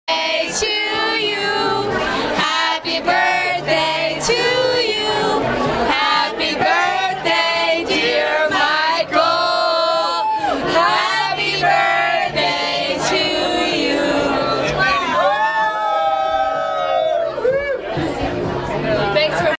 RedCarpetPamela~YRCiTV.com films Michael Capponi's Miami Birthday wishes
RedCarpetProductions~YRCiTV.com films Michael Capponi's Miami Birthday wishes from 220ft. Allures Shadow yacht. Proceeds from this party benefit THE UNITED WAY!
Hire Red Carpet Productions for your next Event, Wedding and Party to film and capture these priceless memories in HD.